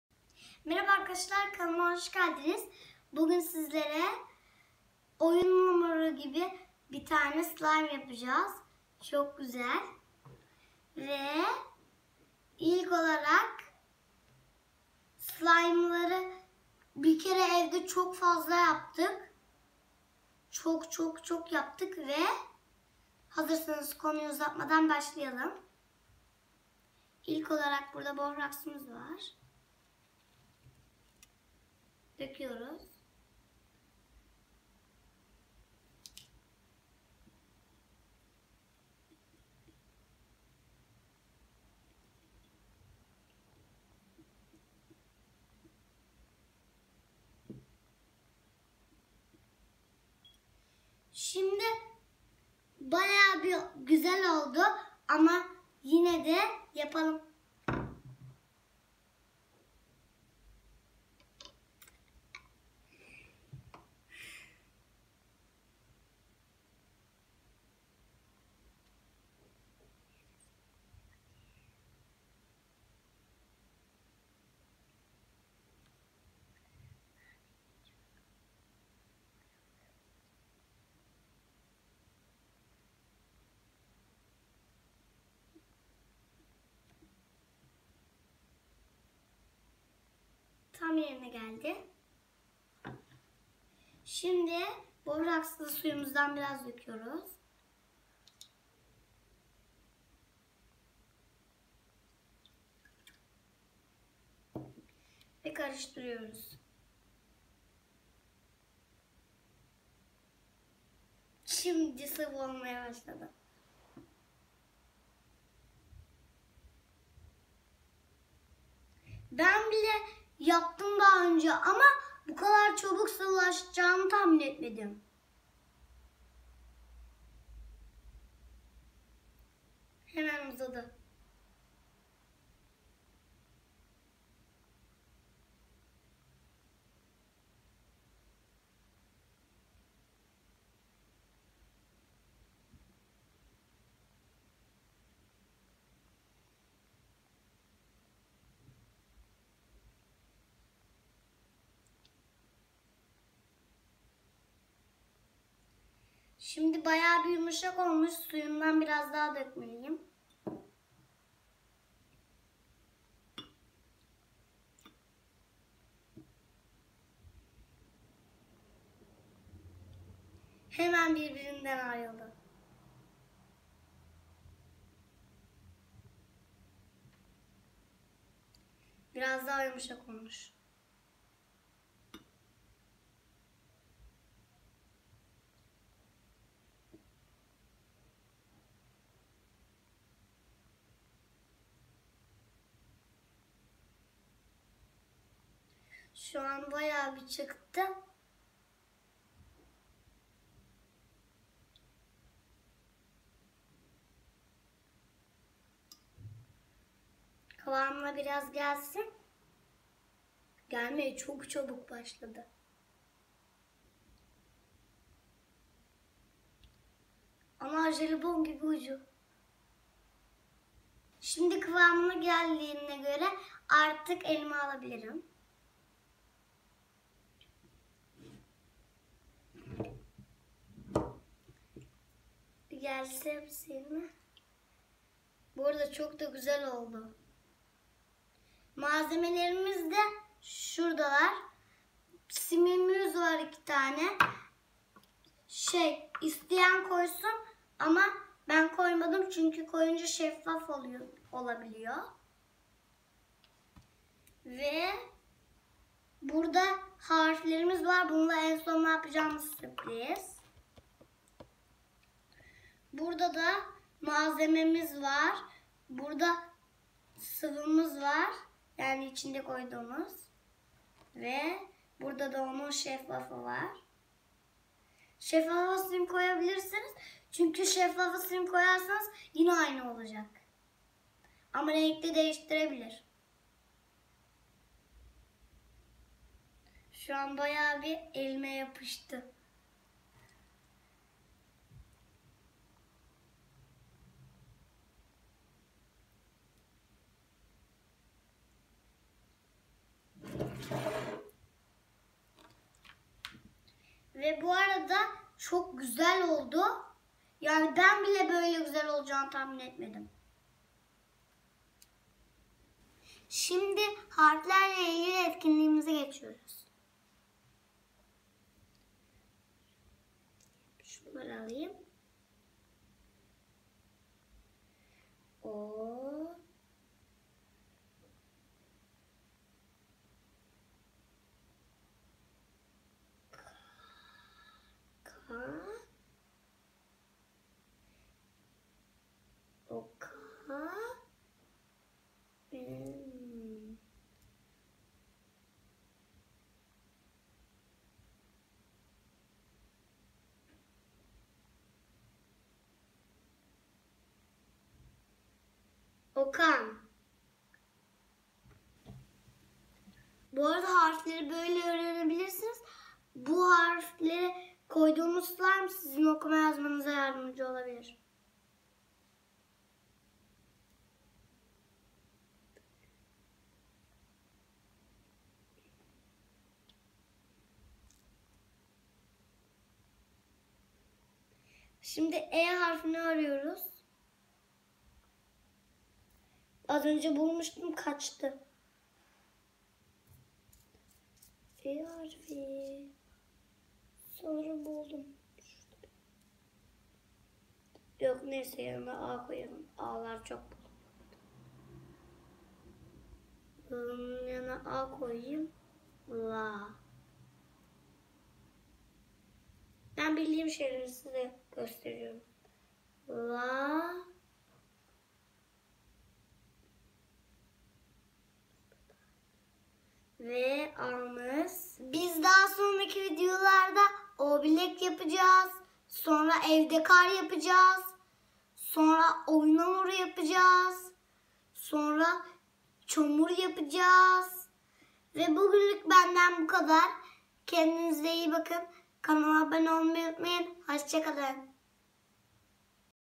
SLIME - ZIPLAYAN OYUN HAMURUNDAN SLIME YAPIMI - OKUMAYI ÖĞRENMENİN EN EĞLENCELİ YOLU: HARFLİ SLIME
Zıplayan oyun hamurundan slime nasıl yapılır? Okumayı öğrenmenin en eğlenceli yolu harfli slime, bizim buluşumuz :)